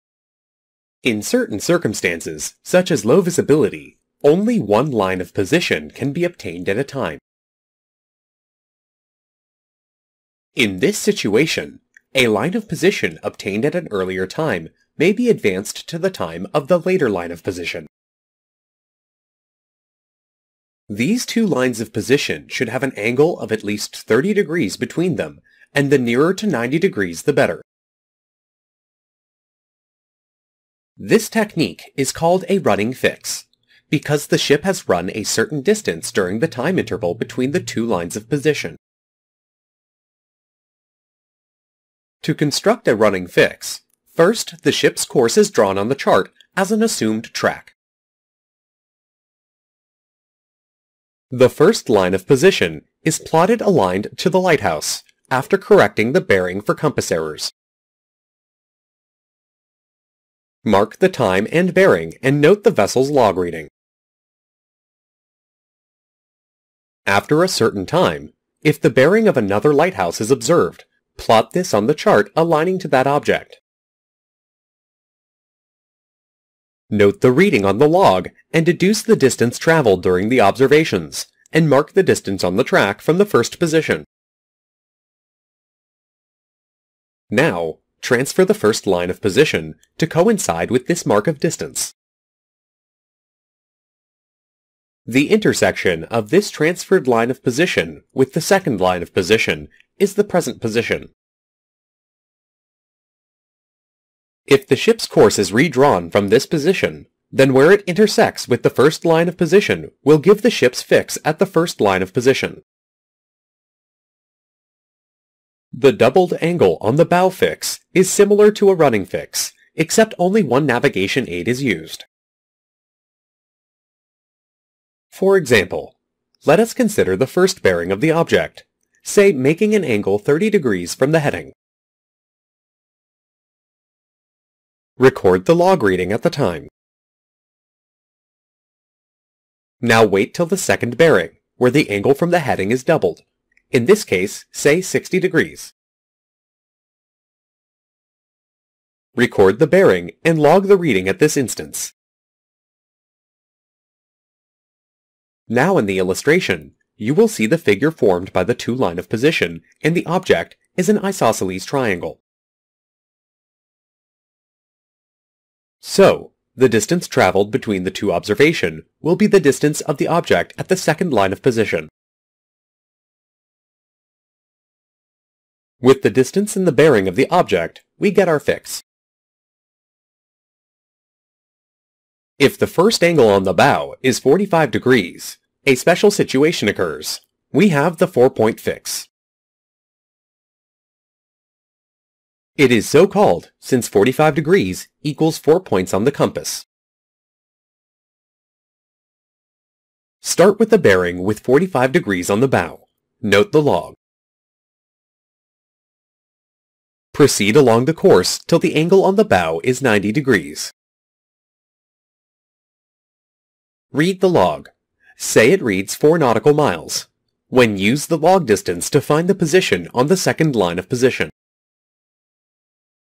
Running Fix | Terrestrial navigation | CHARTWORK BASICS | Merchant navy knowledge
What is meaning of running fix of ship?

Under some circumstances, such as low visibility, only one line of position can be obtained at a time. In this event, a line of position obtained at an earlier time may be advanced to the time of the later LOP. These two LOPs should not be parallel to each other; remember that the optimal angular spread is 90°. The position obtained is termed a running fix because the ship has “run” a certain distance during the time interval between the two LOPs.

It is more commonly used when only one object is available for bearings and there is no means of measuring the range. In this case there is a planned delay between bearings so that the change in bearing will provide an acceptable angle of cut.

The running fix (or transferred position line) is a method to determine your position if you only have one  visual reference point (like a single lighthouse). The methodology is as follows;

As you approach the single point, take a bearing on it with your hand held compass. Make a note of the log, and course being steered.
Once there is an appreciable change in the angle to the object, take another bearing and note the new log reading.
Plot the 1st bearing, and anywhere along this position line, make a mark and from this point, plot the course steered. Measure down this track the distance travelled (the difference between the 2 log reading). and make a second mark.
Transfer your first position line, keeping it parallel, down to your second mark.
Where this transferred line crosses the second compass bearing is your position.
Consider the accuracy of this method? How well was the course steered? How accurate is the log? Was there any tidal streams of leeway?
These can all be factored in to improve the fix, but generally, if the distance between the 2 bearings is not too large, it will be accurate enough.